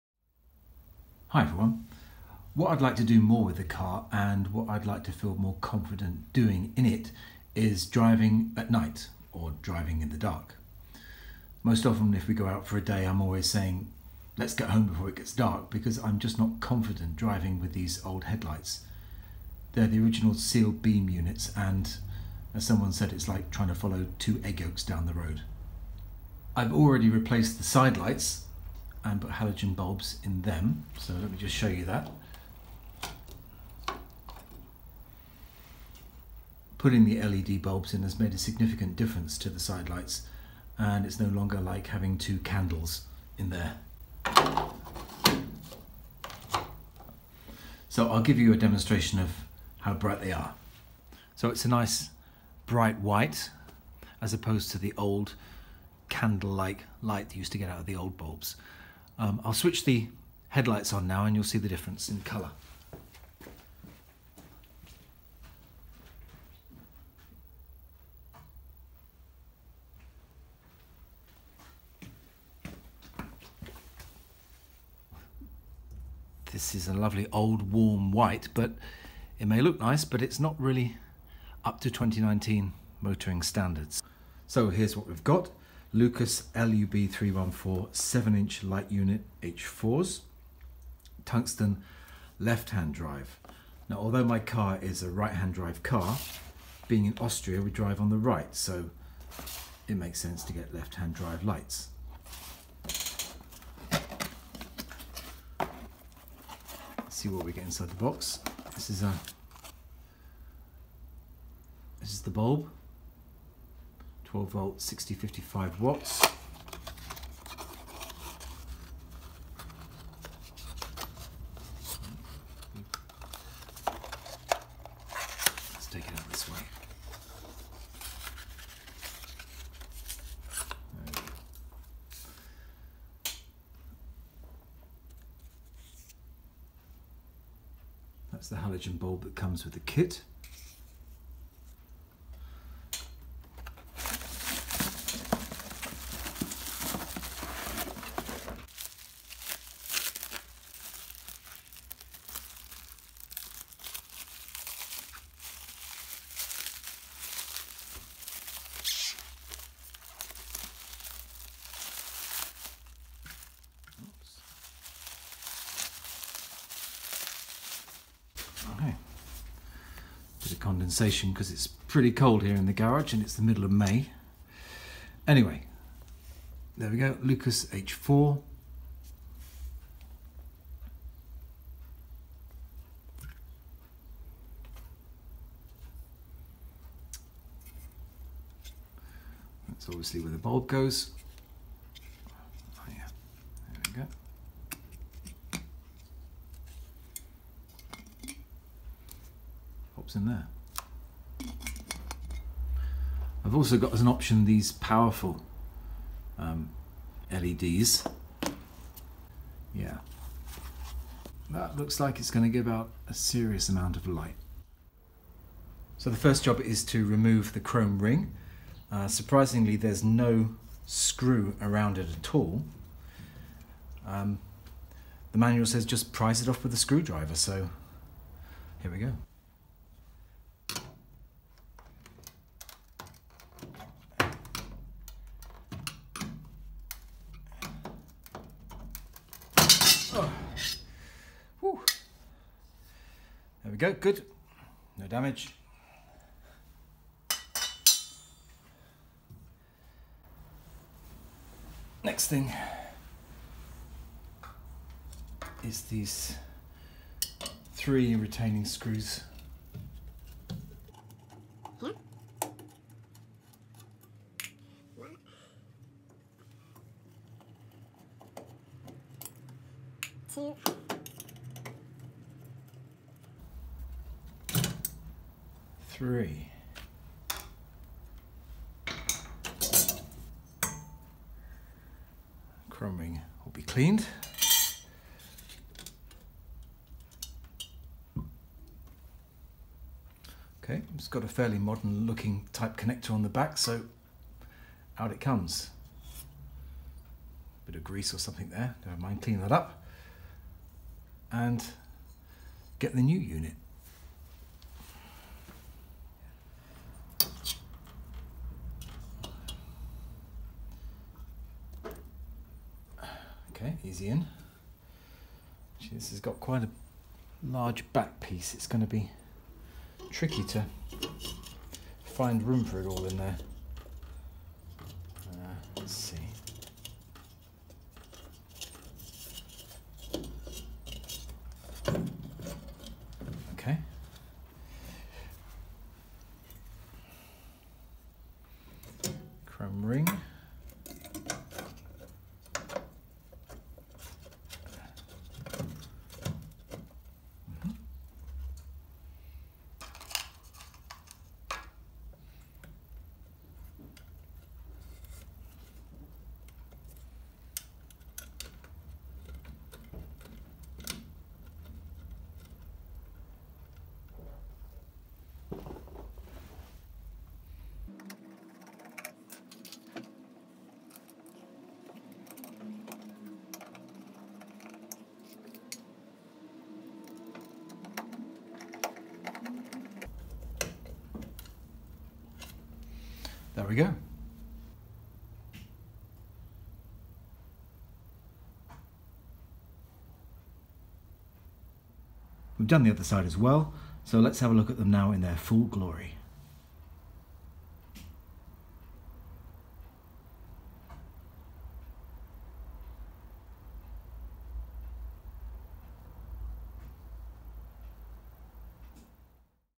Rover P5b LED Headlights Conversion
Time to modernize the Rover's headlights with new LEDs units. A nice easy job!

The exact ones I used are not listed anymore, but the company are super helpful and if you contact them, they'll sort you out.